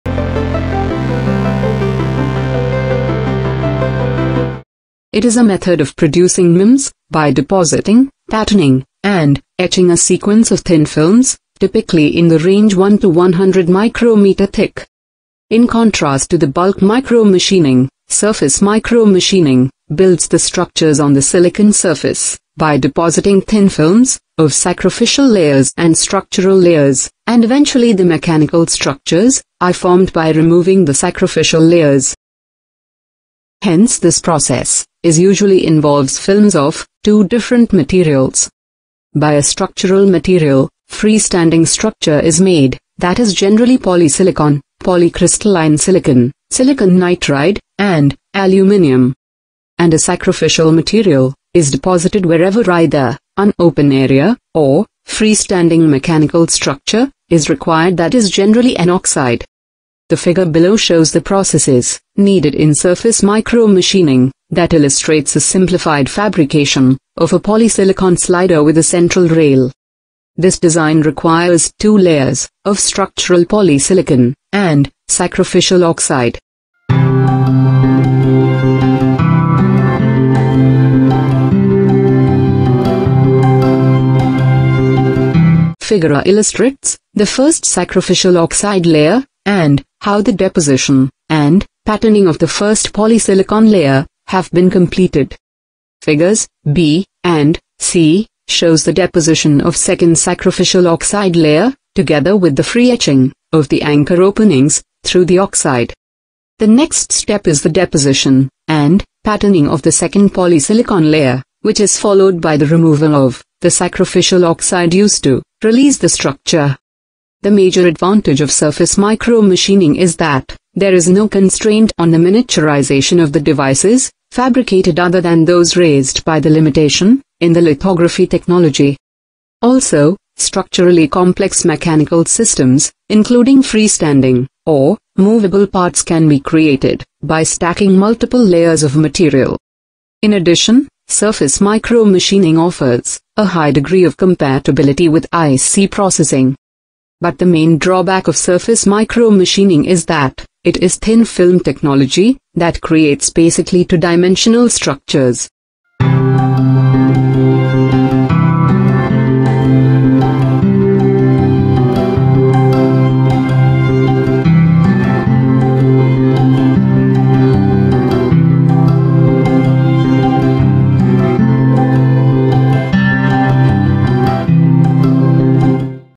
Surface Micro Machining (MEMS Technology) - Micro Electro Mechanical System
It is a method of producing MEMS by depositing, patterning and etching a sequence of thin films typically in the range 1- 100 micro meter thick.
In contrast to the bulk micro machining, surface micro machining builds the structures on the silicon surface by depositing thin films of sacrificial layers and structural layers and eventually the mechanical structures are formed by removing the sacrificial layers.
Hence this process is usually involves films of two different materials. By a structural material, free standing structure is made that is generally polysilicon, polycrystalline silicon, silicon nitride and aluminium. And a sacrificial material is deposited wherever either an open area or free standing mechanical structure is required that is generally an oxide.
the processes needed in surface micro machining that illustrates a simplified fabrication of a polysilicon slider with a central rail. This design requires two layers of structural poly silicon and sacrificial oxide.